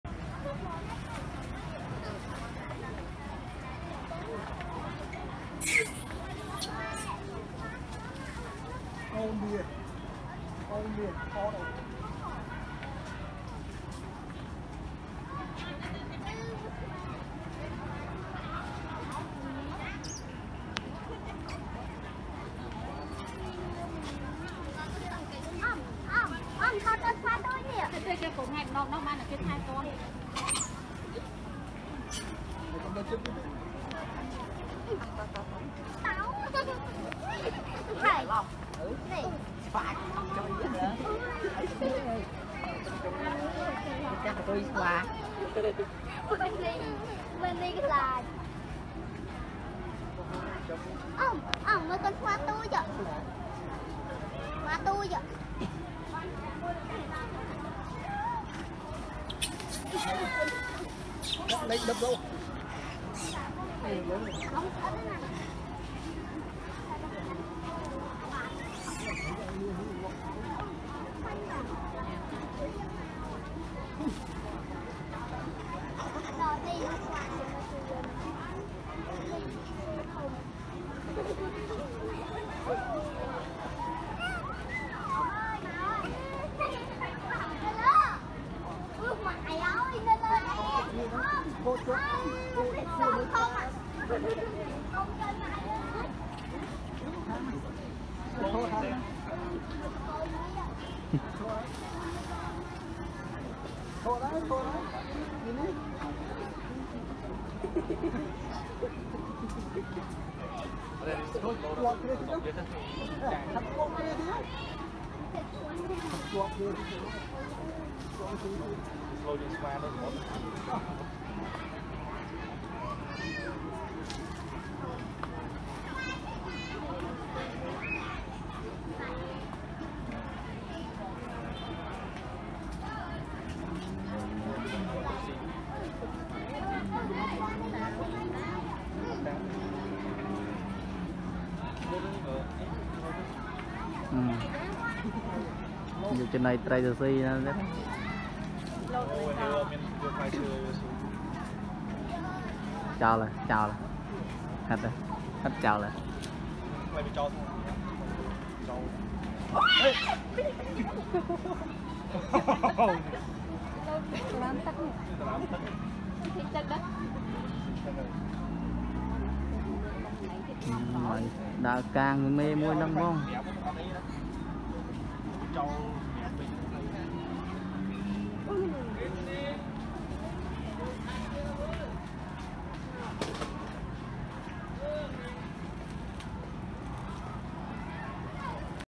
Many monkeys are very happy on tree | Life Monkeys | Kun Wildlife TV
I am very thankful that you came to my channel. Thank you so much and wish you all the best! Please be happy with Monkeys video to relax.